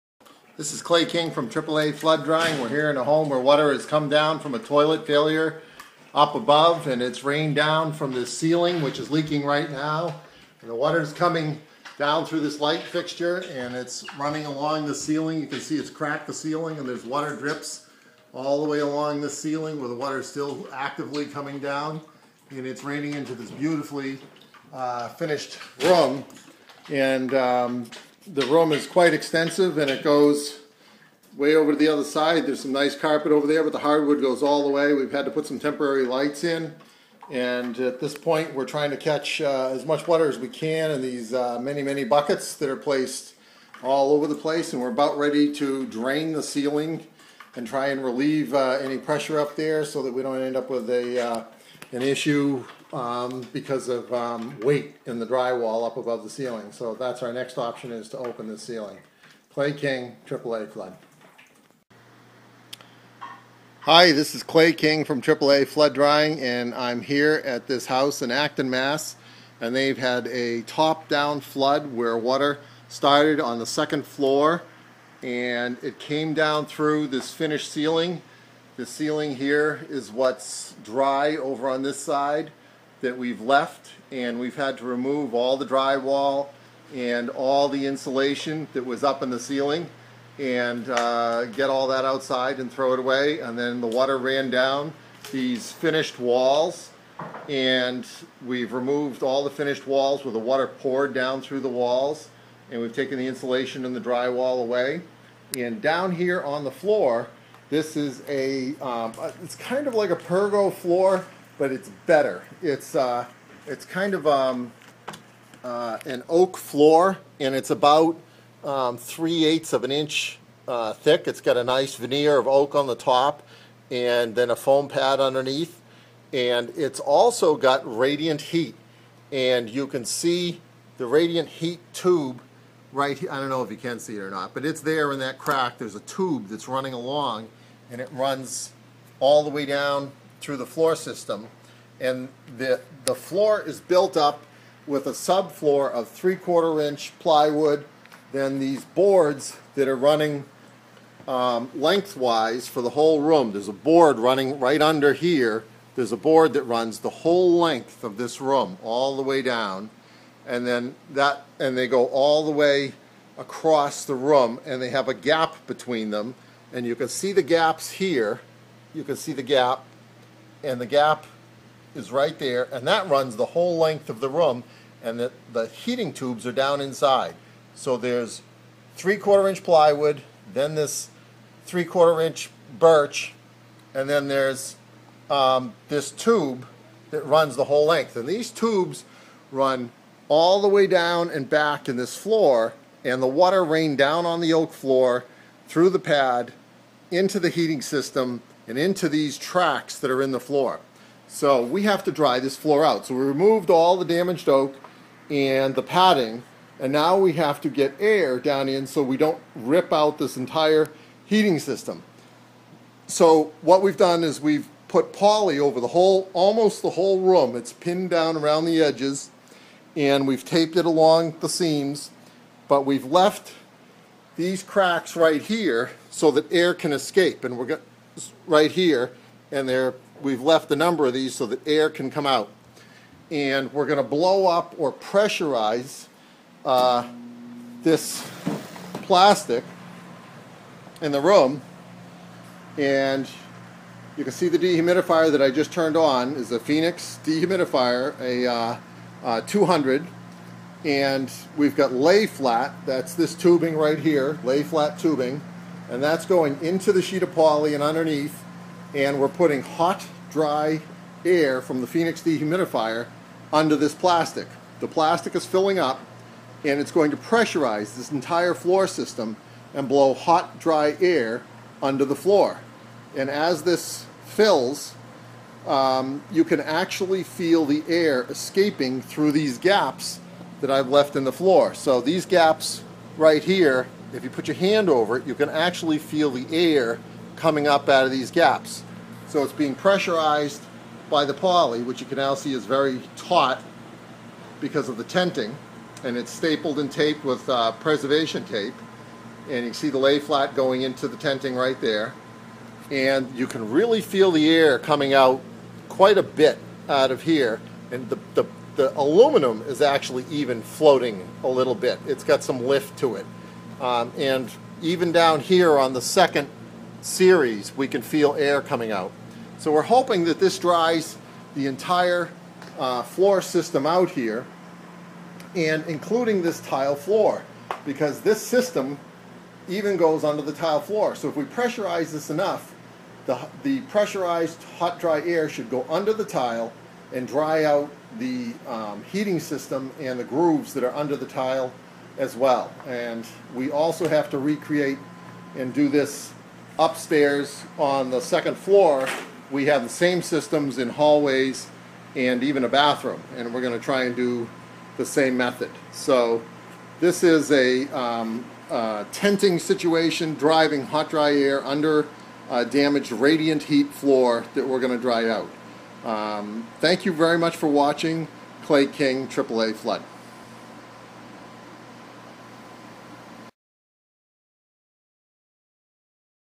Drying Water Damaged Radiant Wood Floor, Massachusetts
Drying a radiant wood floor damaged by water. Water ran under a hardwood floor into the radiant heat system. The goal was to save the expensive radiant heat system and not have to remove and replace the system. The wet damaged hardwood floor and padding were removed to access the radiant heat grooves. A tenting system was set up to dry out the damaged floor. Hot dry air was forced into the grooves in the floor to dry the wet wood subfloors.